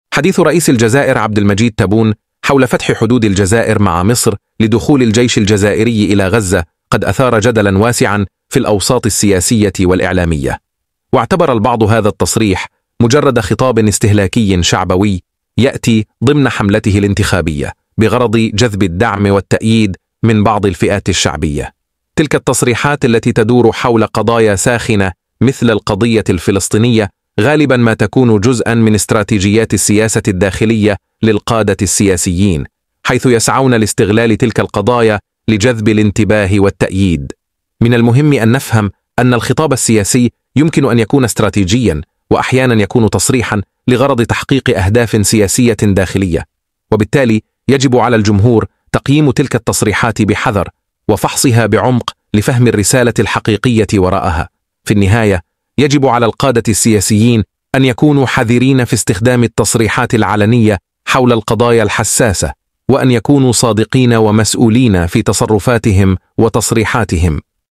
حديث الرئيس تبون وطلبه مصر فتح حدودها لإدخال الجيش لغزة:هل هو تحليق خيالي؟
في هذا الفيديو، نستكشف تأثير وجدية تصريحات الرئيس تبون حول فتح الحدود مع مصر بغرض دعم غزة، ونفحص ما وراء الكواليس لهذه الخطوات.